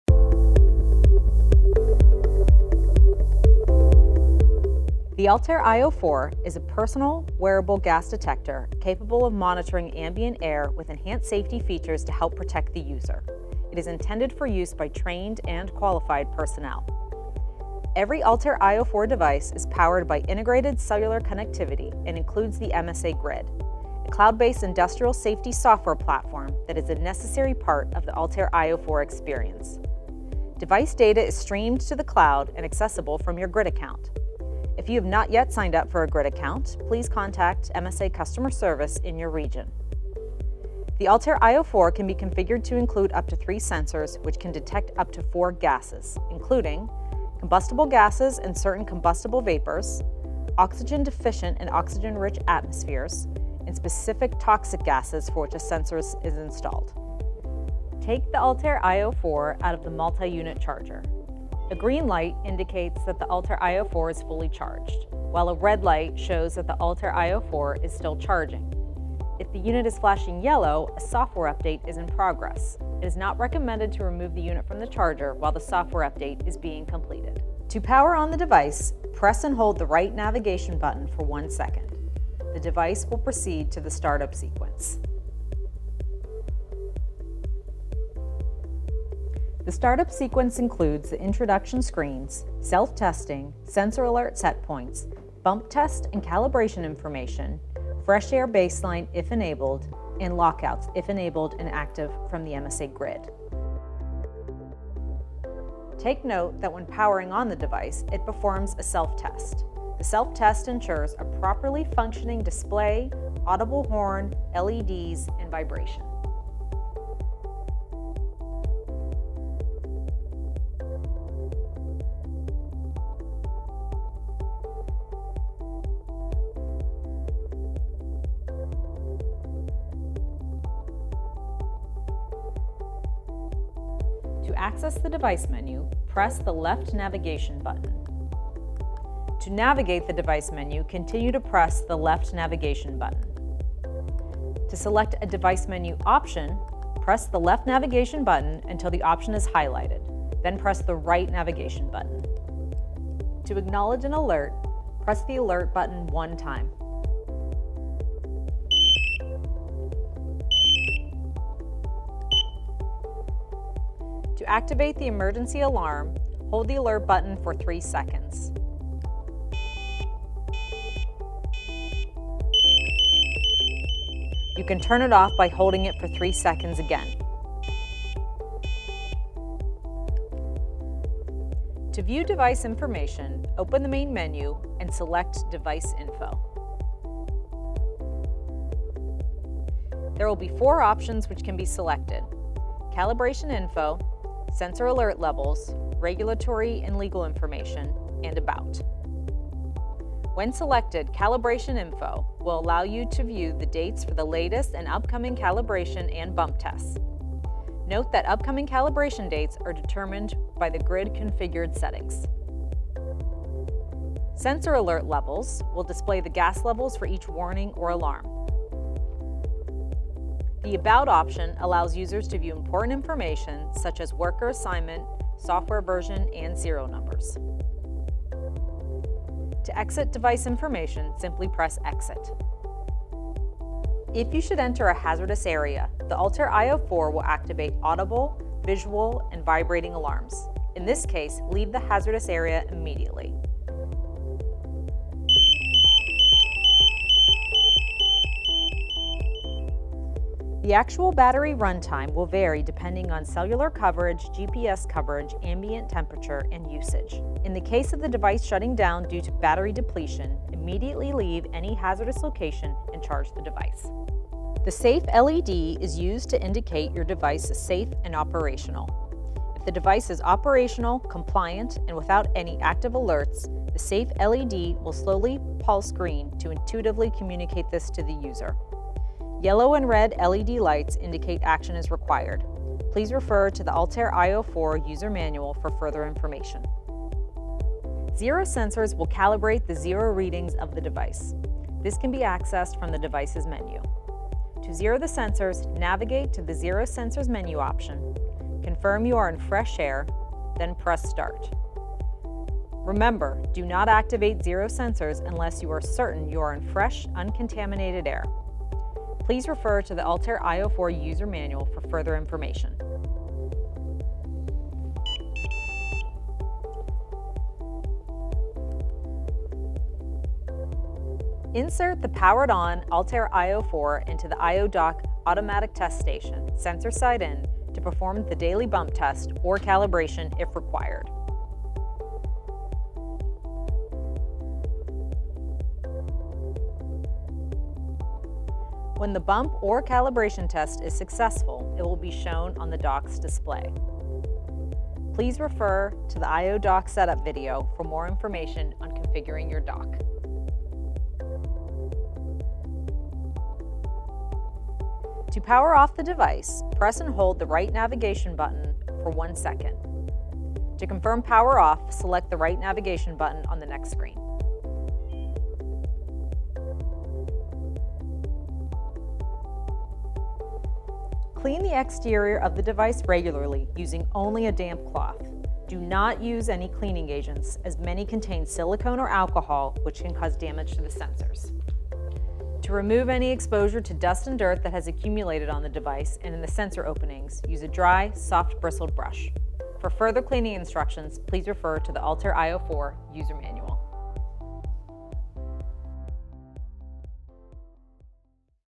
ALTAIR io™ 4 Care and Use​
Learn basic steps for daily care and use of MSA’s ALTAIR io 4 multi-gas detector. The video covers basic information which each user may require in daily handling of this instrument, including start-up sequence, gas and battery alarms, and  bump testing with the ALTAIR io Dock test system.